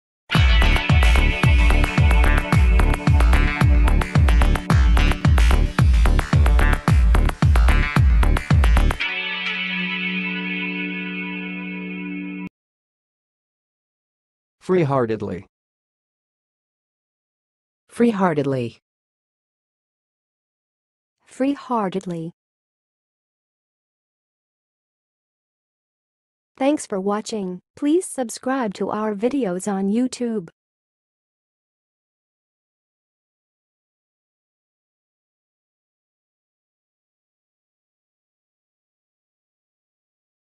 How to say "freeheartedly"! (High Quality Voices)
Watch in this video how to say and pronounce "freeheartedly"!